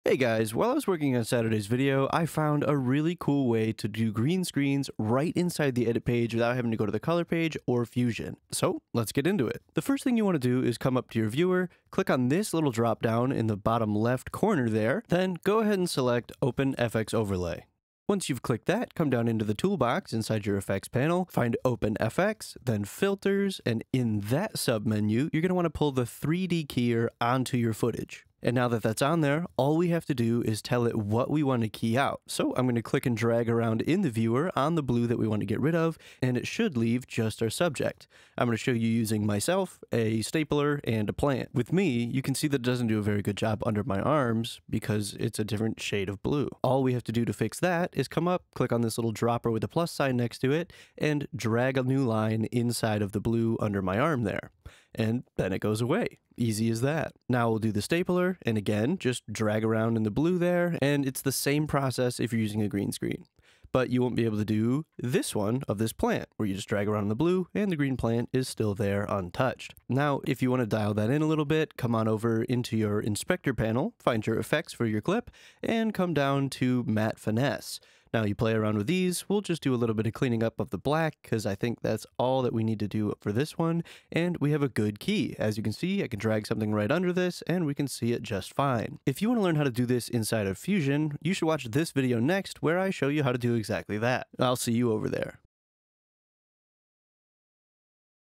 Possibly the Fastest Way to Remove a Green Screen (Or a Blue Screen).
I found this cool thing while working on Saturday's video, at first I wasn't planning to make a video about it but I think it's actually pretty awesome so I decided to show you guys how to do it! Super fast and pretty good green screen action! Enjoy!
If you like the content and want to help offset the people who use adblockers it'd be awesome if you'd consider buying me a coffee!